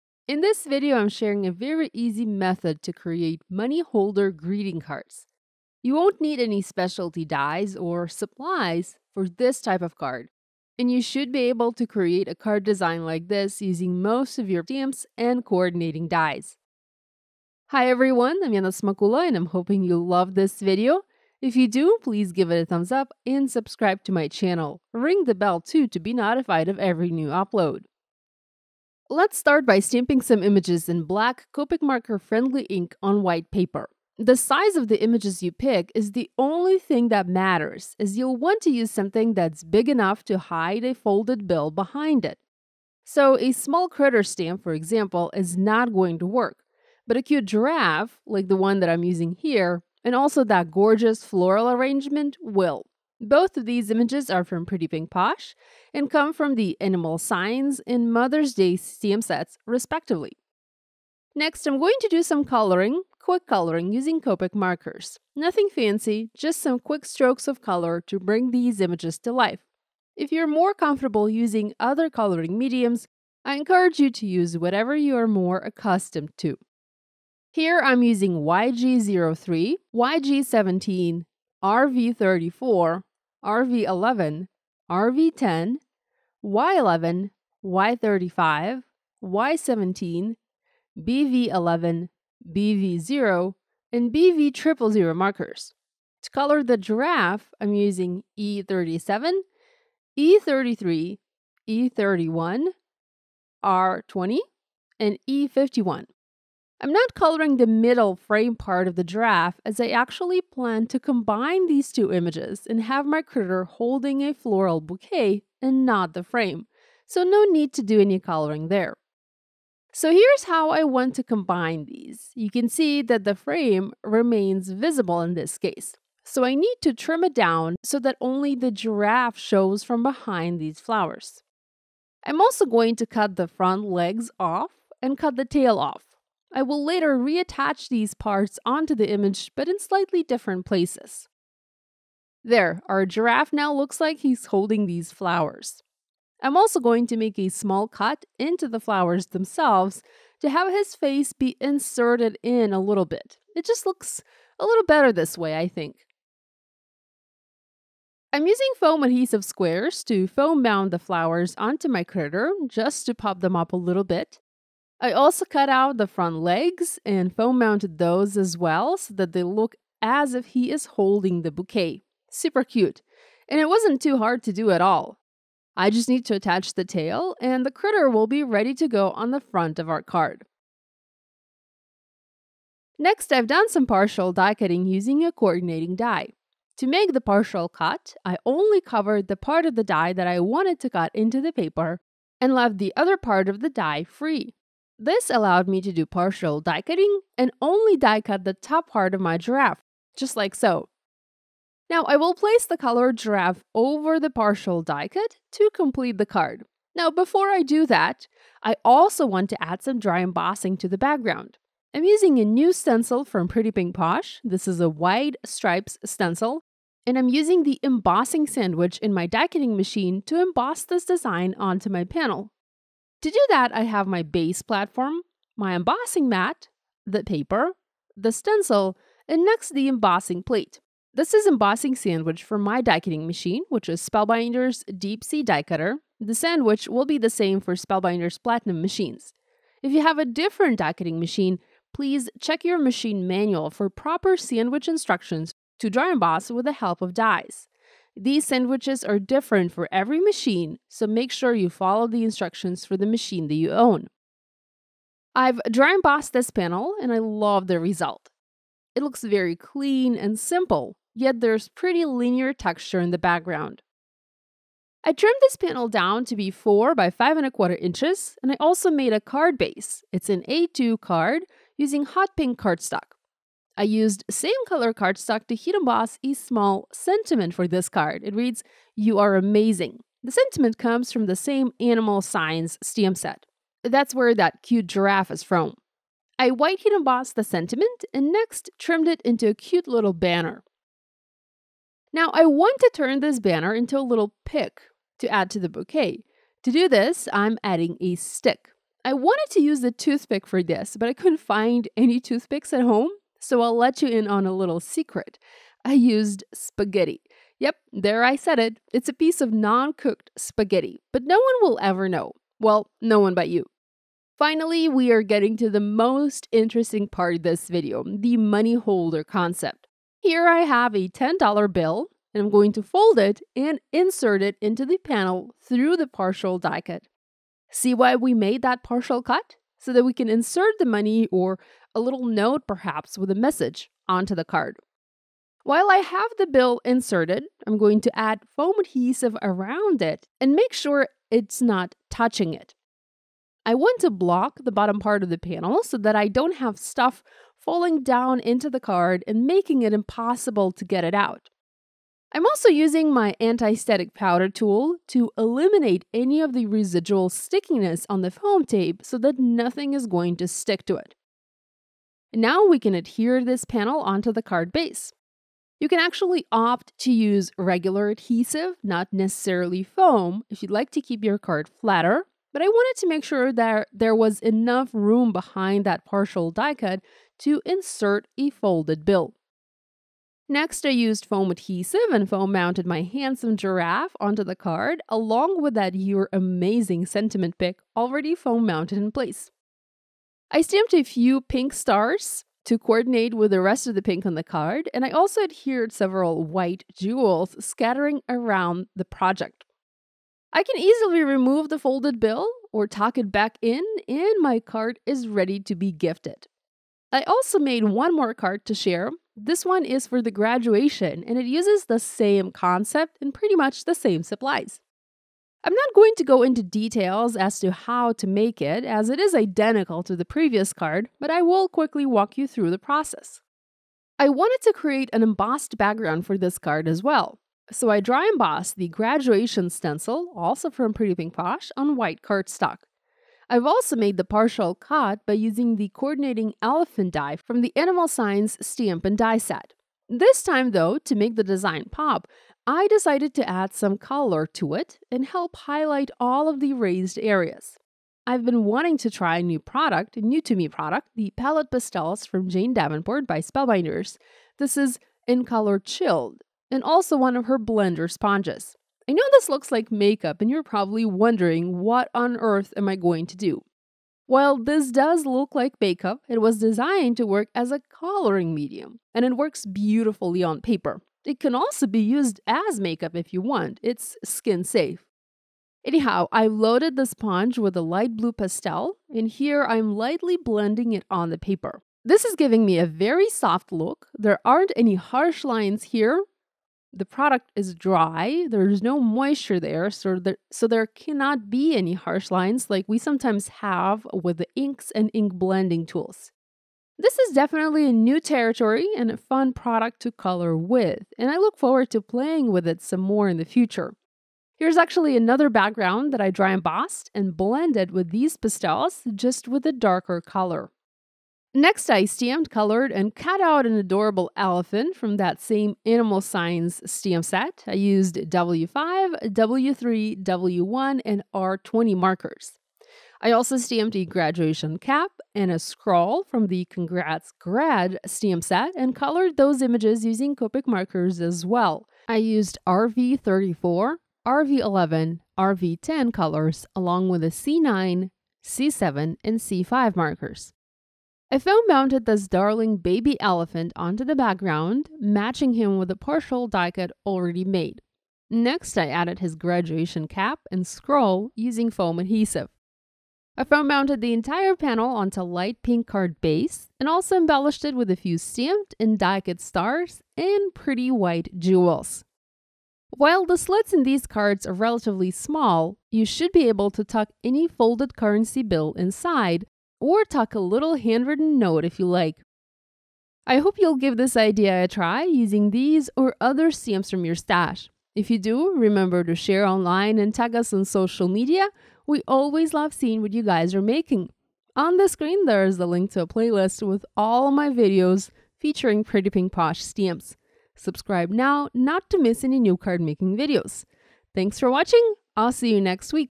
How to Make Money Holder Cards (for Graduation & More) - Easy Handmade Cards
Sharing a very easy method to create money holder greeting cards. You won’t need any specialty dies or supplies for this type of card and you should be able to create a card design like this using most of your stamps and coordinating dies.

• Neenah 110 Lb Solar White Cardstock
• Hero Arts White Detail Embossing Powder
• Hero Arts Heat Embossing Gun
• Versamark Watermark Emboss Ink Pad
• EK Success Powder Tool Applicator
• Mini Misti Stamping Tool
• Tonic Deluxe Adhesive Nuvo Glue
• Crystal Ninja Crystal Katana Pick Up Tool
• Chilled Palette Pastel Set from Making Faces by Jane Davenport
• Blend In Blender Sponge Set from Making Faces by Jane Davenport